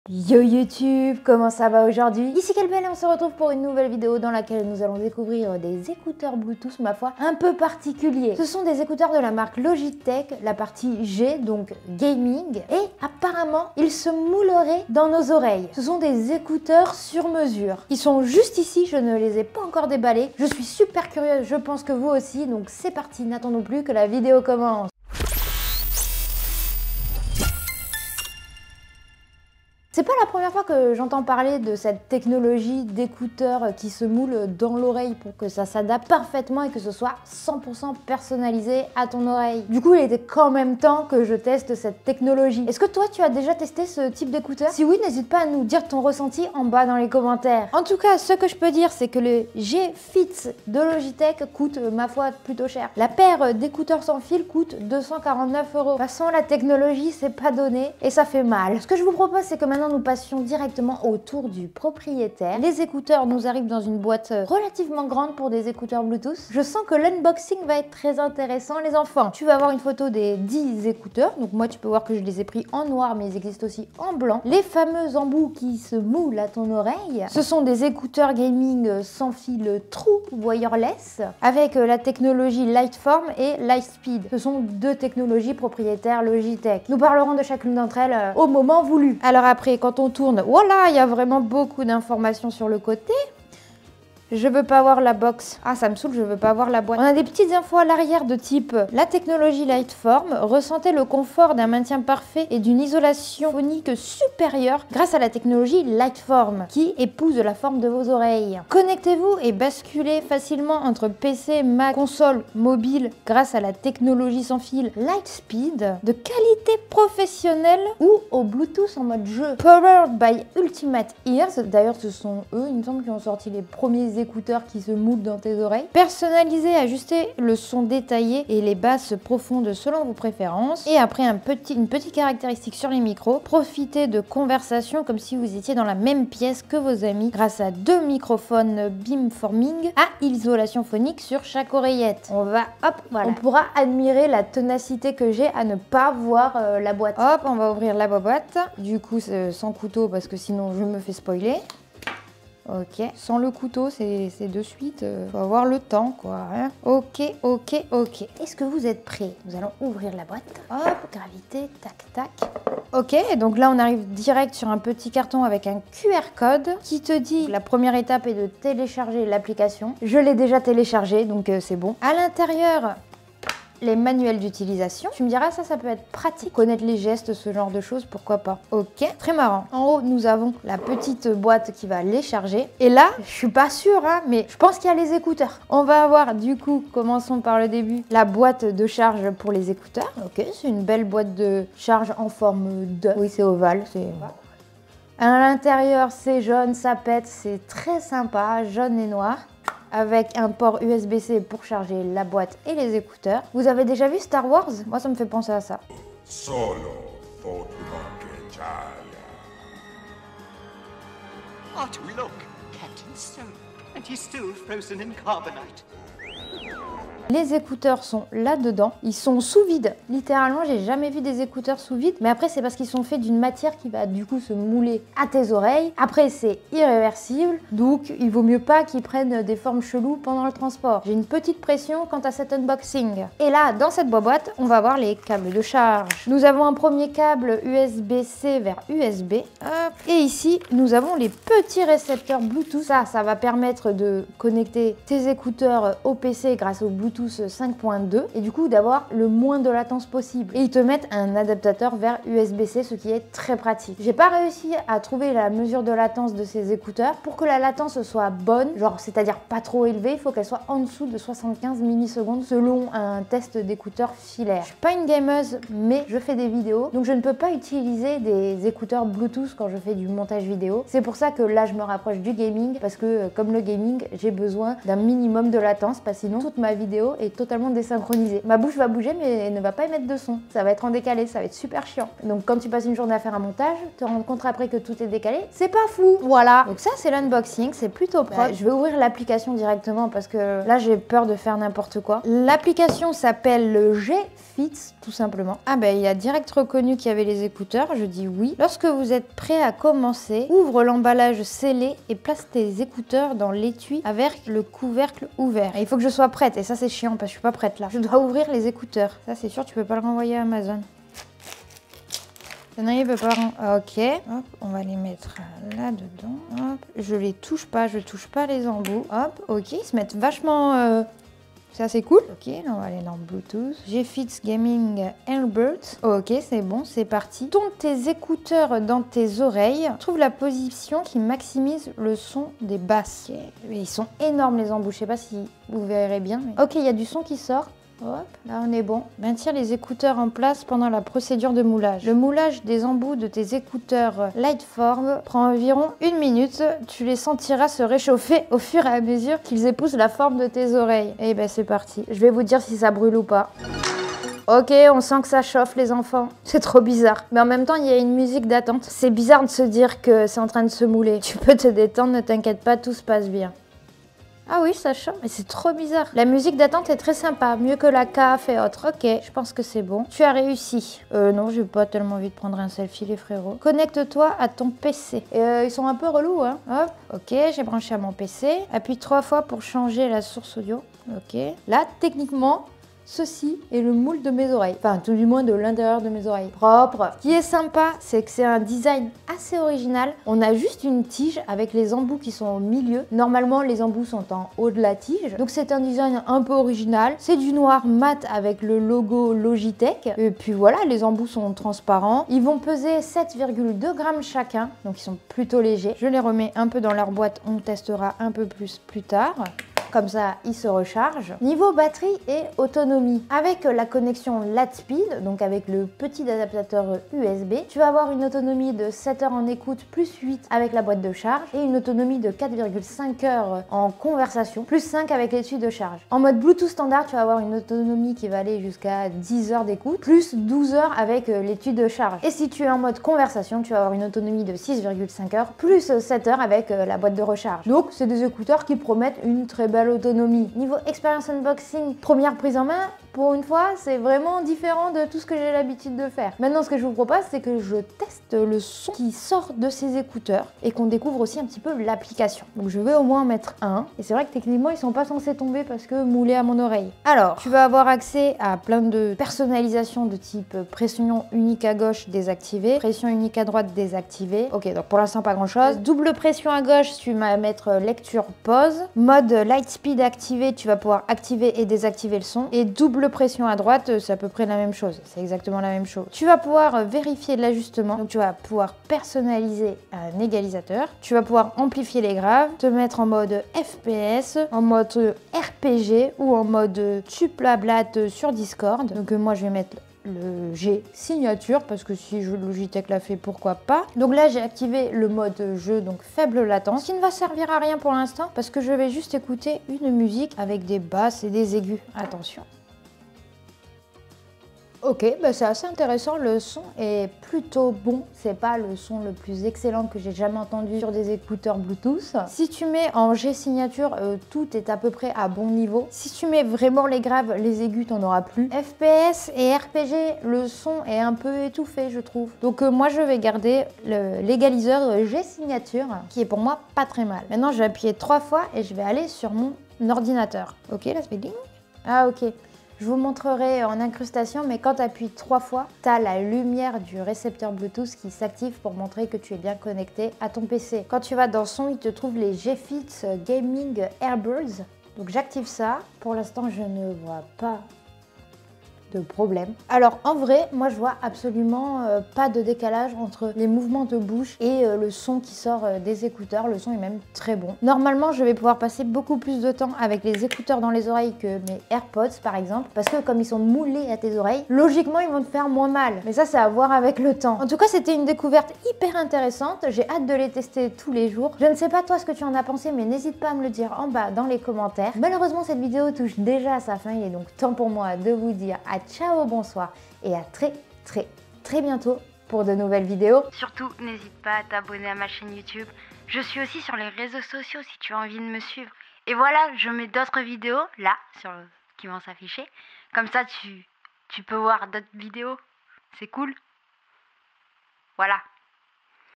CES ÉCOUTEURS SONT AUTO AJUSTABLE ! - Logitech G Fits
Des écouteurs gaming sans fil qui s’ auto moulent à la forme de tes oreilles! Intriguant non ?

Logitech G Fits

MON MATÉRIEL
►Sony A6400
►Objectif 16-50 mm
►RODE Wireless Go
►SmartLav Micro cravate
►Lumière principale
►Deuxième lumière
►Mon pied OneDesk